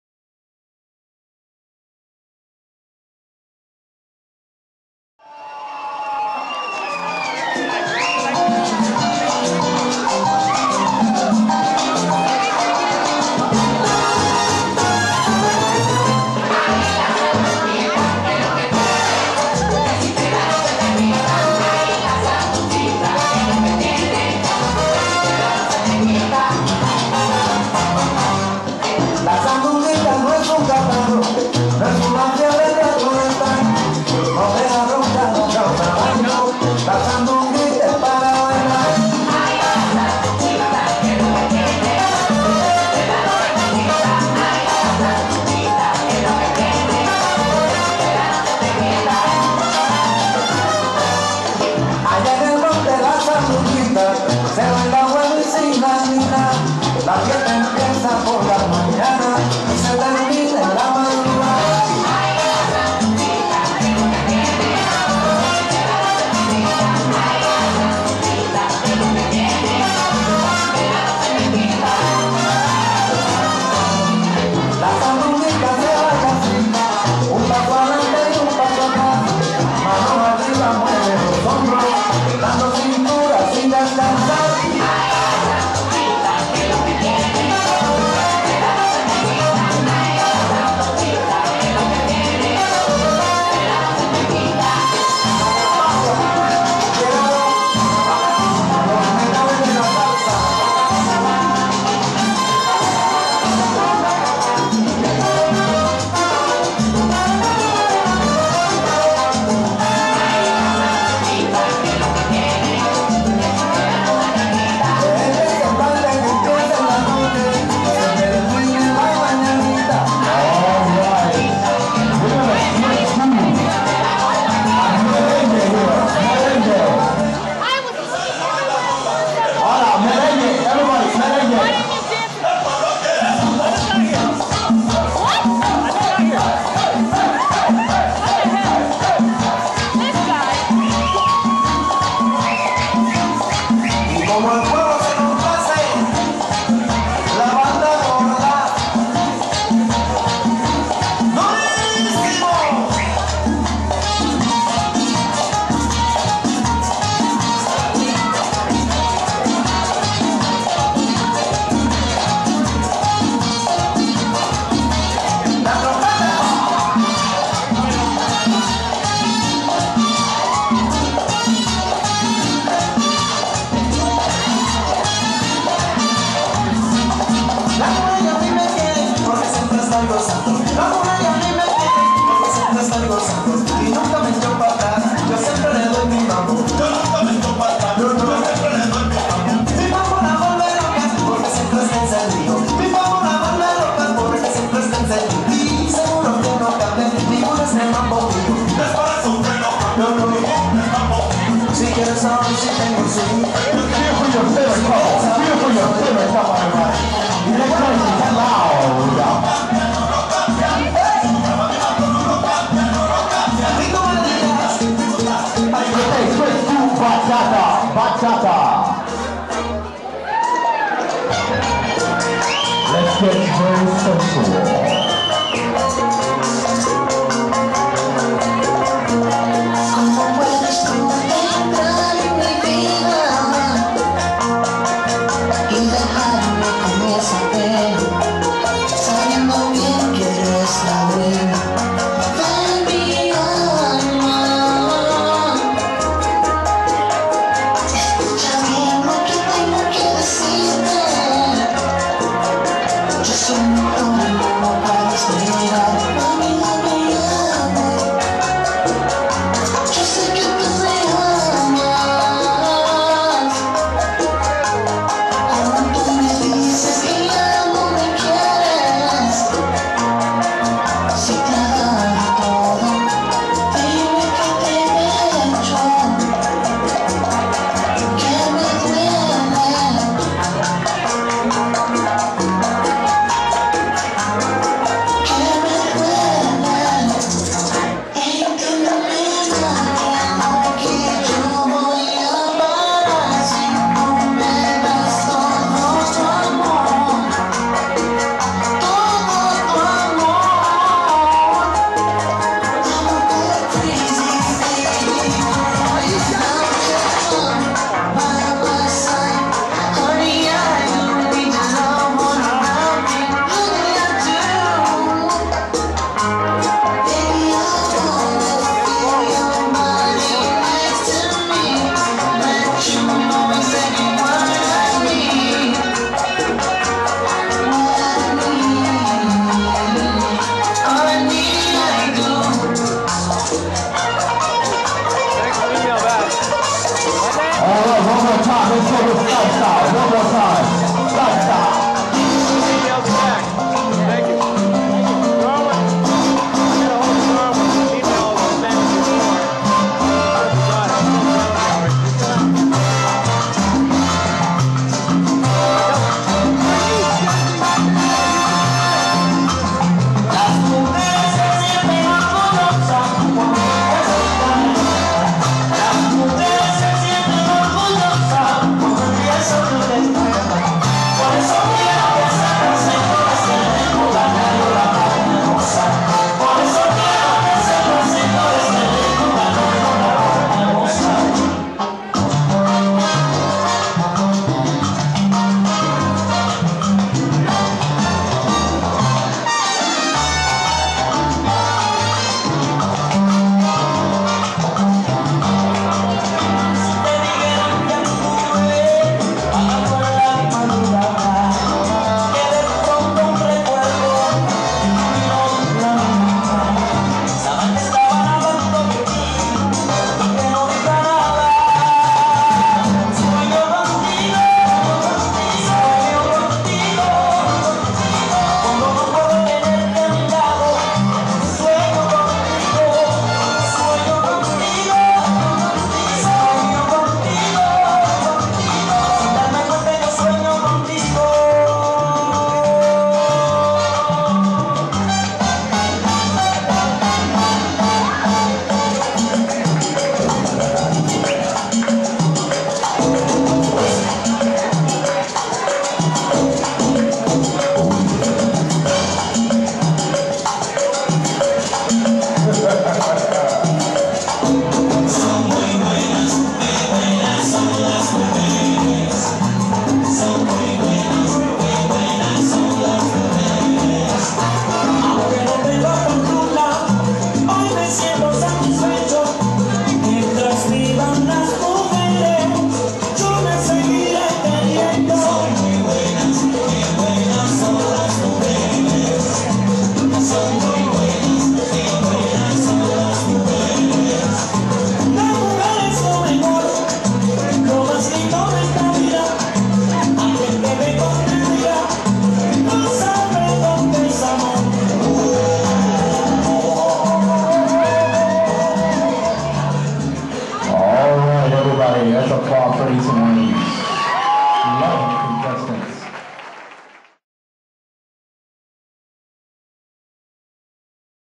Salsa Cozzy Latin Dance Contest Round 1
Round 1 of JC Entertainment's Salsa Cozzy Latin Dance contest. 1/26/07